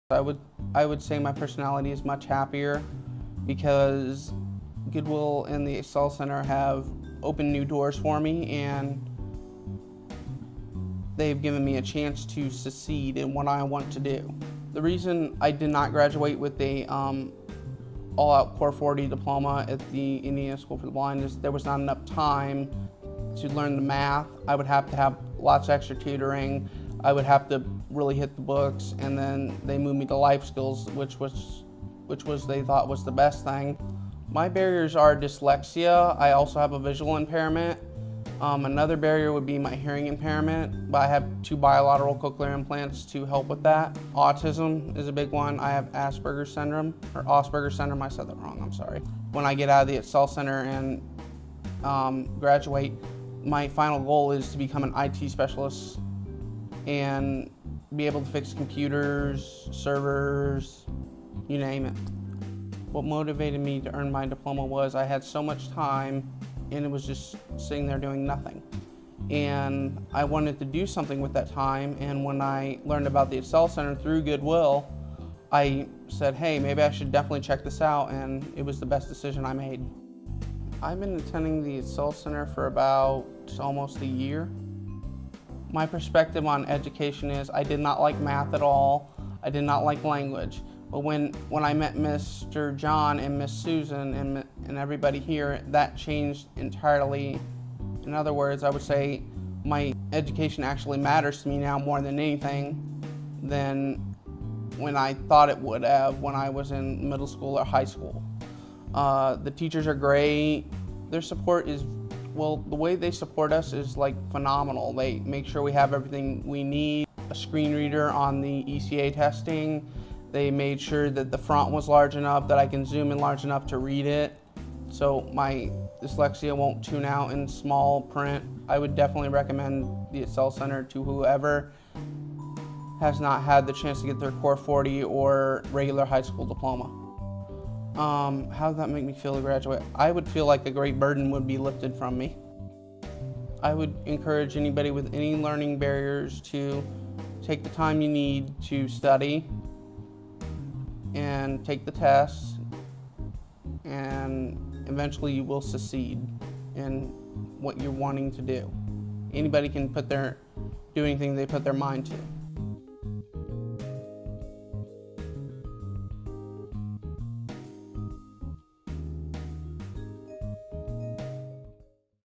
Meet Jacob Barrett
Jacob Barrett may have barriers to education, but you wouldn't know it based on his success record. Working his way to graduating with his core 40 diploma and on into to a career in IT.